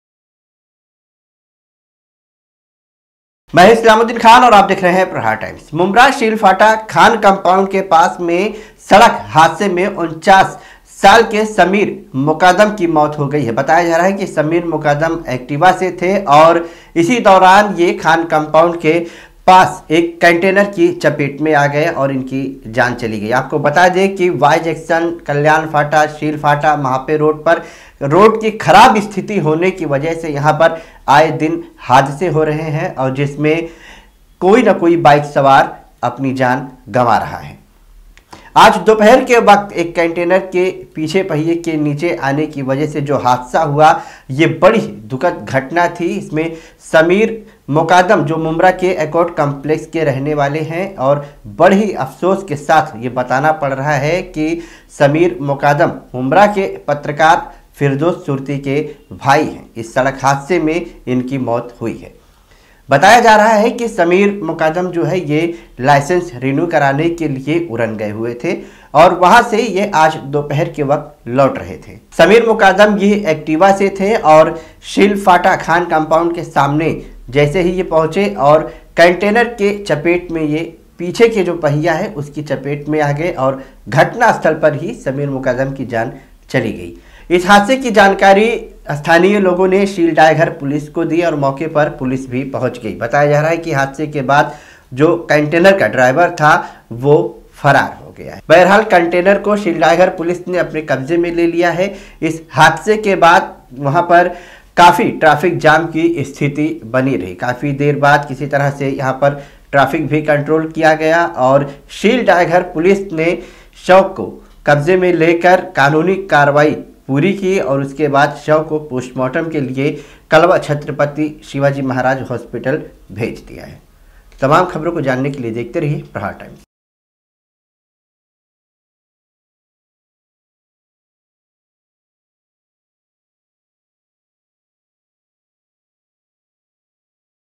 शीलफाटा पर समीर मुकादम की कैंटनर की चपेट में आने से मौत || Islamuddin Khan || Aapka Prahar Times
शीलफाटा पर समीर मुकादम की कैंटनर की चपेट में आने से मौत || Islamuddin Khan

मुंब्रा के शिलफाटा खान कंपाउंड के पास एक्टिवा सवार समीर मुकादम की कंटेनर की चपेट में आने की वजह से मौत हो गई है पुलिस ने कानूनी कार्रवाई पूरी कर शव को पोस्टमार्टम के लिए भेज दिया है